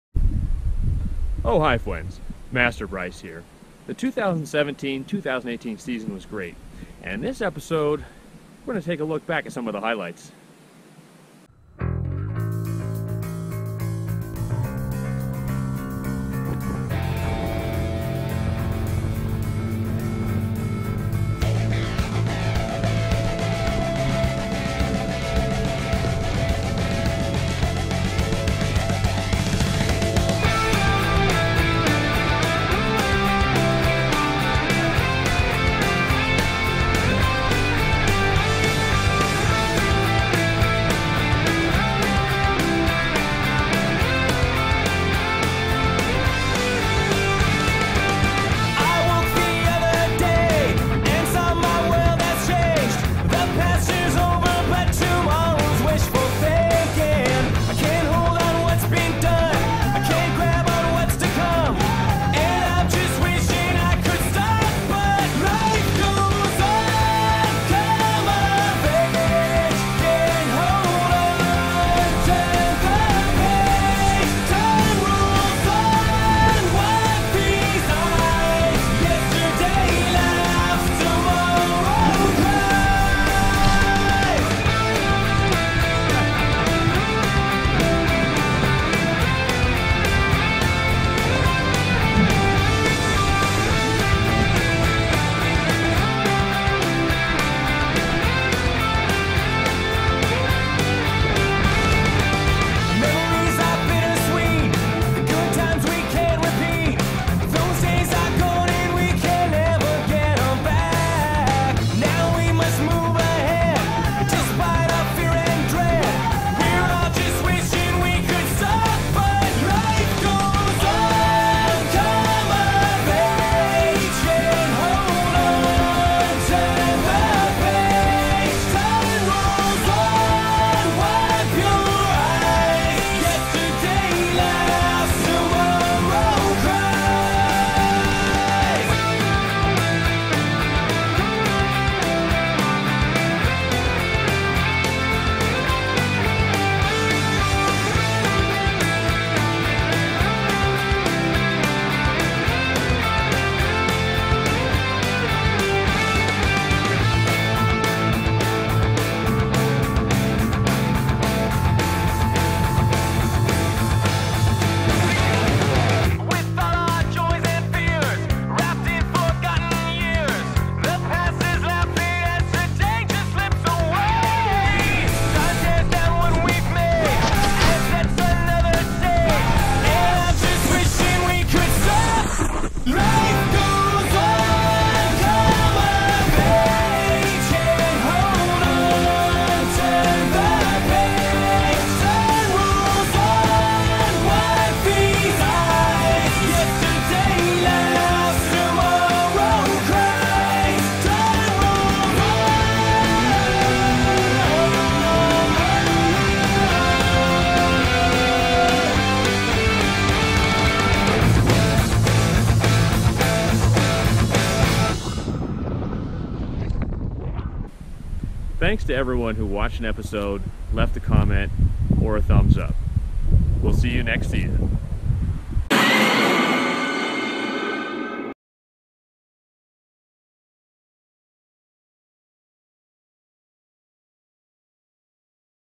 Reflecting on the 2017 / 2018 Winter Season
The 2017 / 18 winter season was a thin slice of heaven for us, and we hope it was for you as well.  Here's to another great season!

Resort: Crystal Mountain, Washington, USA
Featured riders: Blue Steve and Justin-san